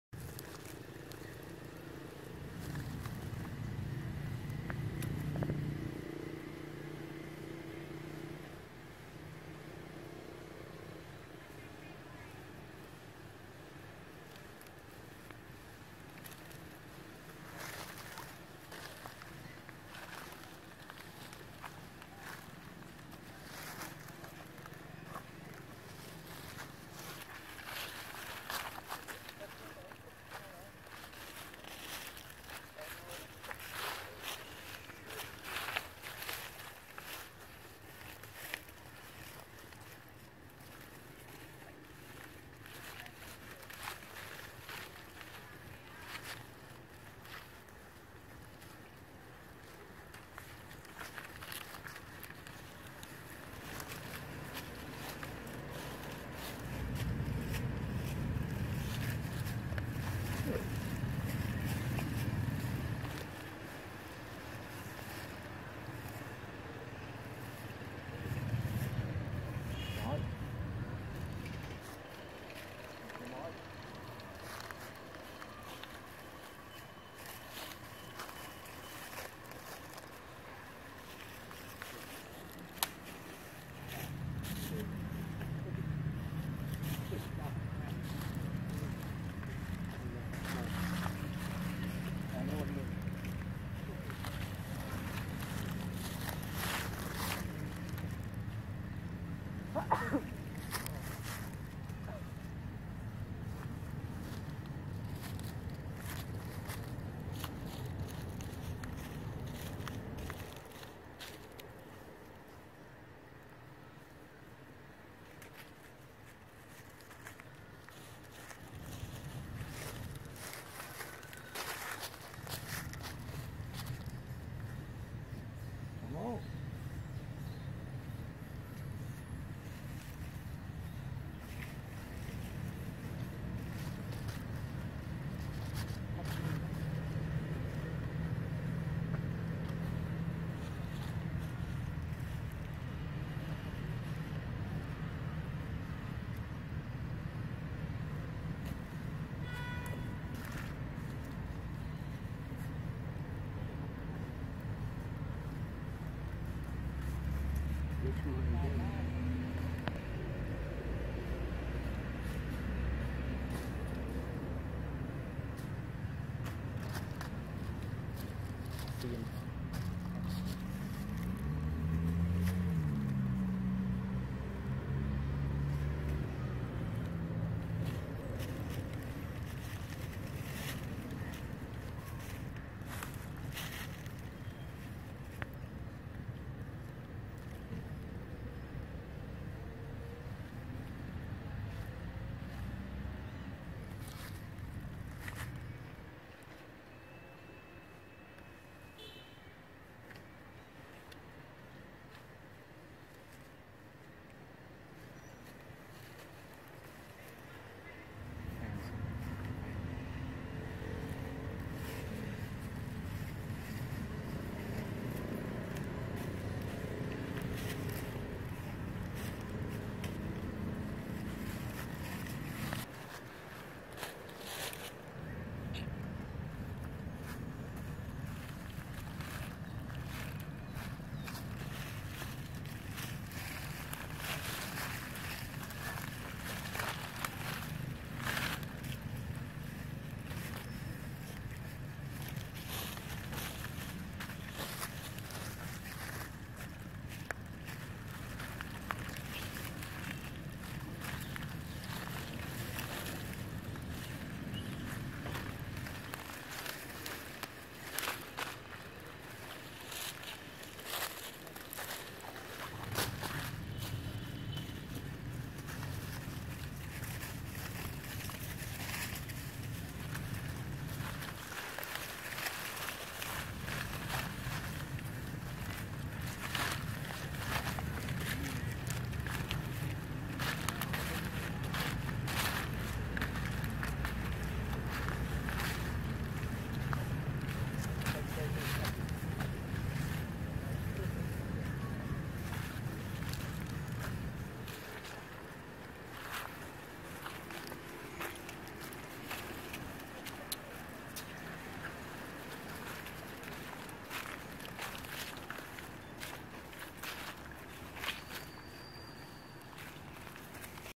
Beautiful baby monkey Viola holding her mother | How beautiful baby monkey Viola as her age
Baby monkey Viola was born on 12\19\2018 her mom is Violet a monkey in Amber group.

If you love watching monkey videos  we will upload more.